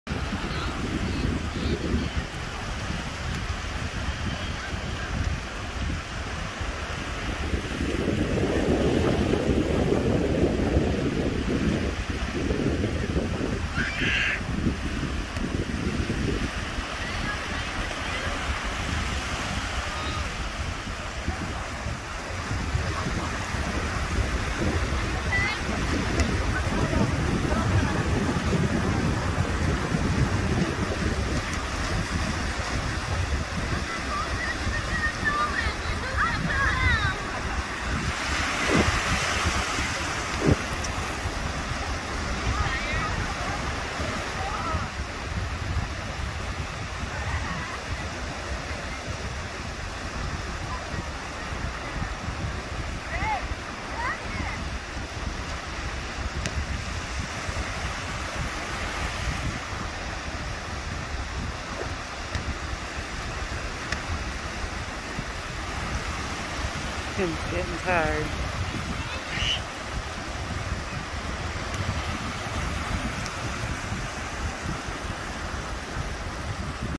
family fun @ the ocean
the family enjoying some good down time we had a blast